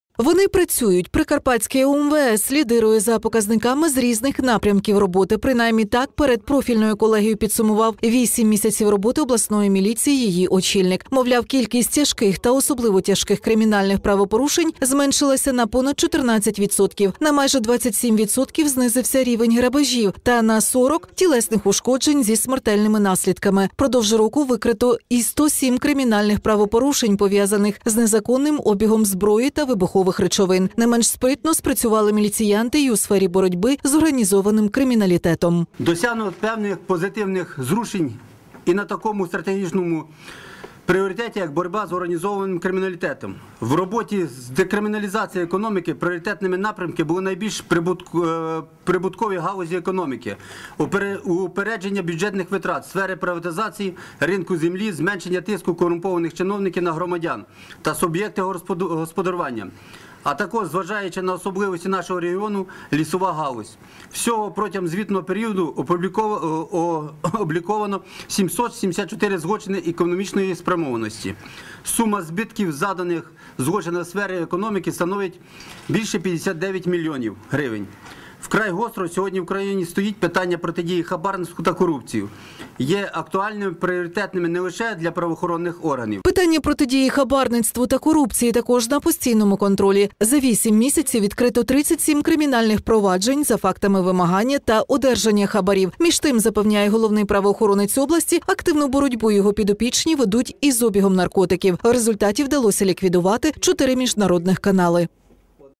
Прикарпатське УМВС лідирує
Принаймні так перед профільною колегією підсумував 8-м місяців роботи обласної міліції її очільник.Канал 402